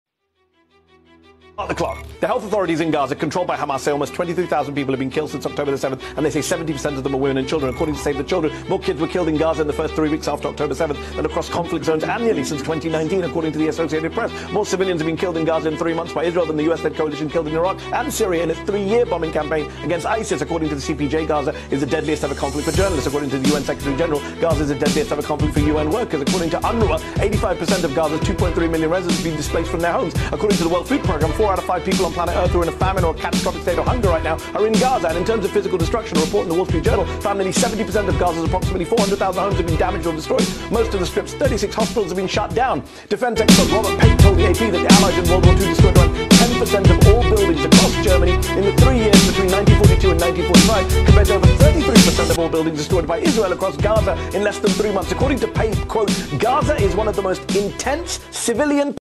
صحفي بريطاني أمريكي يلخص غزة في 60 ثانية
An American journalist explains in 60 seconds what happening in Palestine since of 7th October
صحفي أمريكي يشرح في 60 ثانية ما يحدث في فلسطين منذ 7 أكتوبر
كيف يشرح صحفي عن واقع الحياة في غزة في 60 ثانية قبل مغادرة القناة؟